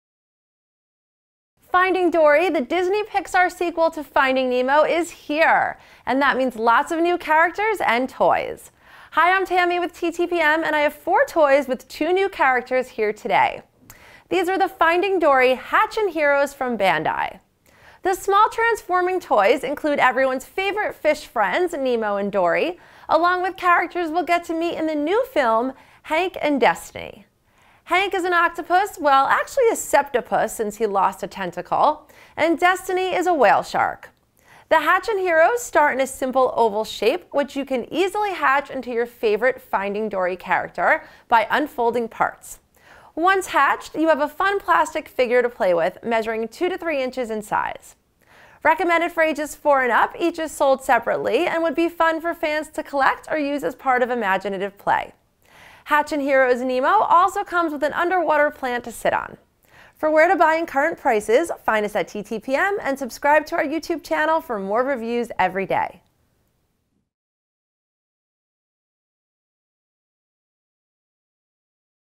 Finding Dory Hatch 'n Heroes from Bandai
Disney/Pixar Finding Dory Hatch ’n Heroes transforming toys. Finding Nemo sequel. Fish friends Nemo, Dory, Hank, and Destiny. Octopus, septopus, whale shark. Oval shape hatch into favorite Finding Dory character plastic figure to collect or use as part of imaginative play. For full review and shopping info

Product Info:
Based on the Disney/Pixar sequel to Finding Nemo, Finding Dory Hatch ’n Heroes are two- to three-inch transforming toys that include Nemo, Dory, Hank, and Destiny. The toys start in a simple oval shape, which you can easily hatch into a fin, plastic figure by unfolding parts.